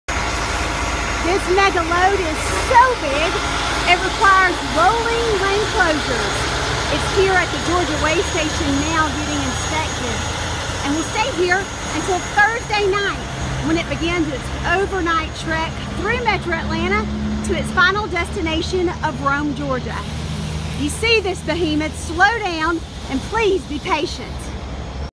Now Habersham
Georgia Department of Transportation District One Communications Officer, Teri Pope, shares video and words of caution about the mega load traveling through Georgia June 10-12, 2015.

The 285 ton, 266-foot long turbine rotor will travel on a 31-axle transport from the SC/GA border to the new General Electric storage facility in Rome, GA. It arrived in Georgia on June 10 and is expected to arrive in Rome around 2am on June 12.